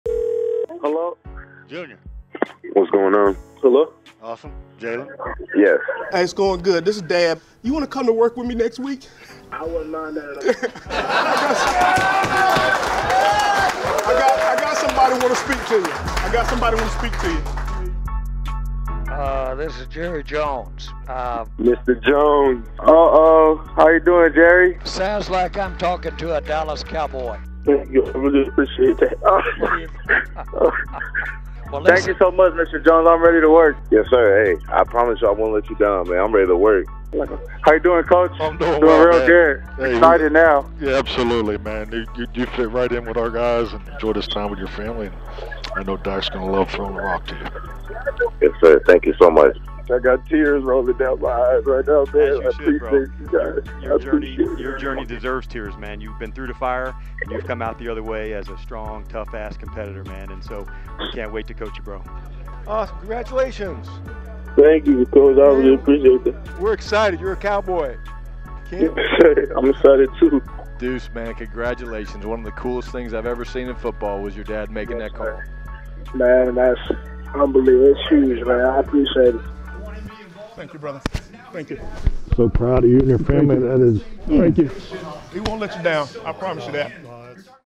Secret Audio: Compilation | NFL Draft | Dallas Cowboys 2023
Listen to a variety of exclusive audio of the calls made to Dallas Cowboys picks during the 2023 NFL Draft.